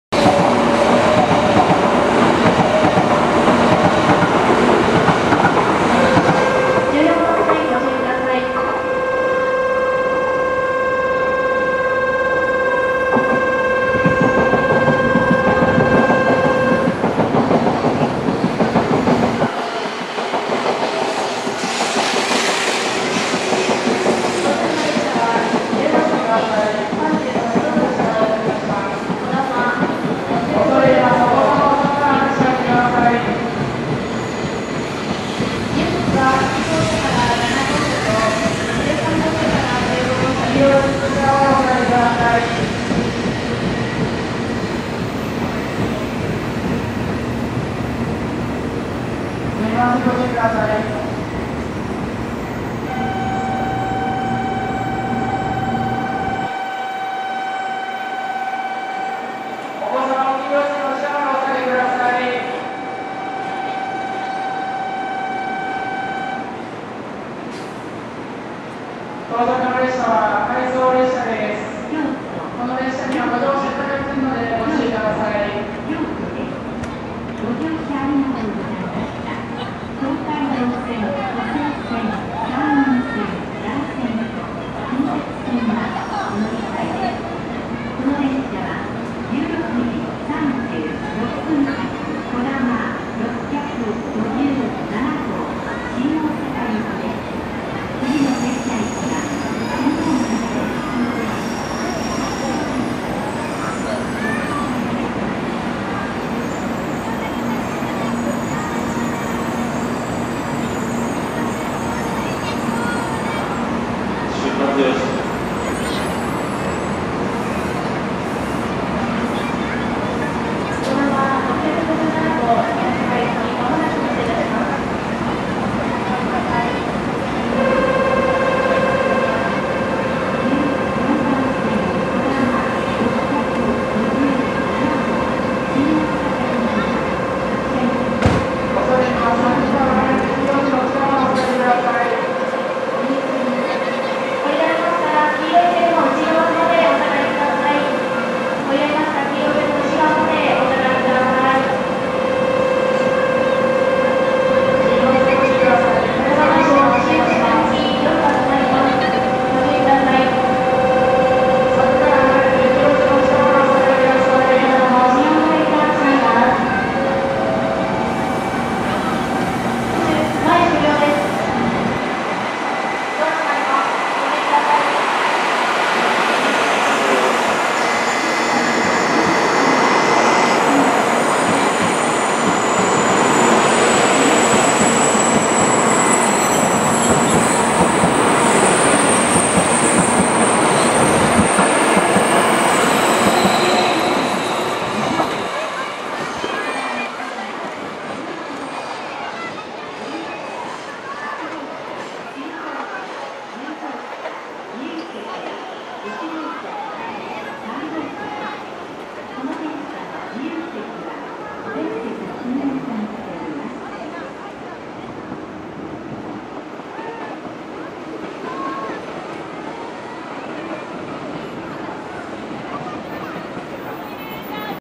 【京都】ドクターイエロー！けっこう過密ダイヤ!? 2013/08/07
のぞみ134号が16:32に発車後に、すぐドクターイエローが16：34に到着、16：36に発車していきました。